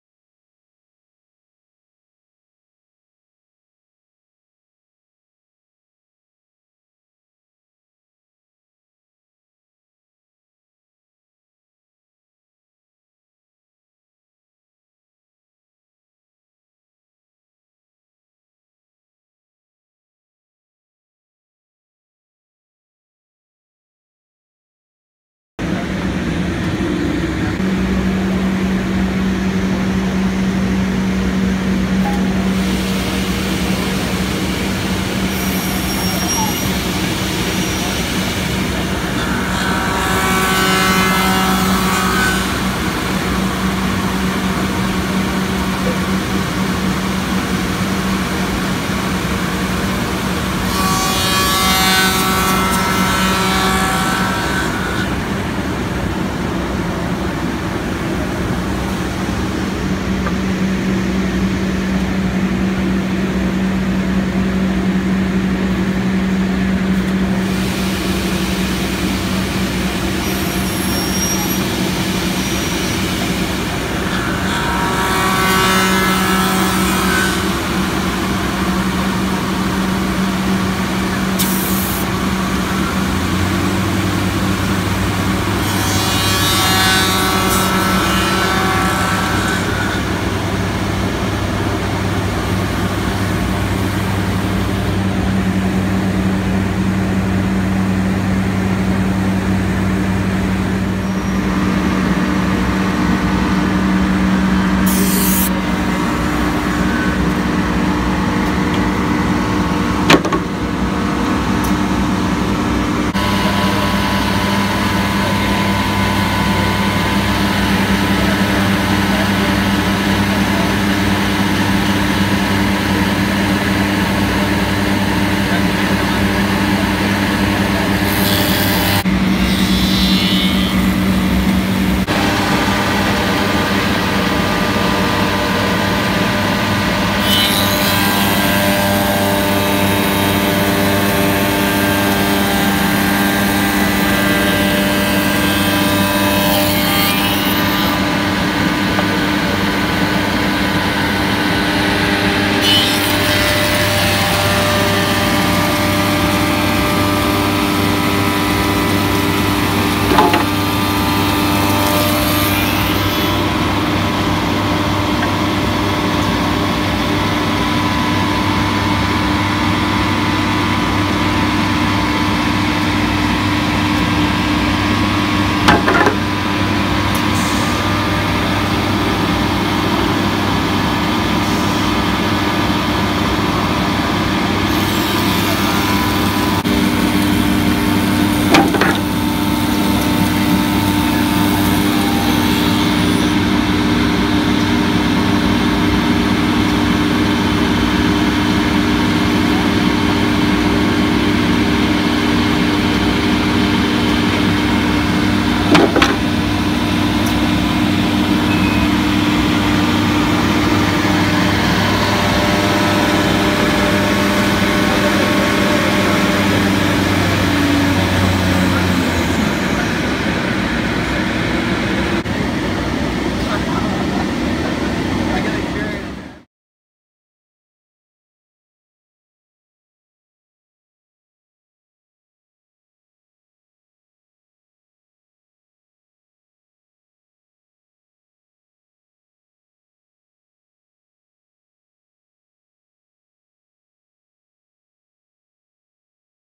SOUKUP window and doors machines at AWFS 2017
Soukup America Inc. has presented for first time at AWFS 2017 Las Vegas show the machining of doors and windows on the CRAFTER machine. It is one of the most efficient systems suitable for small and mid sized joinery.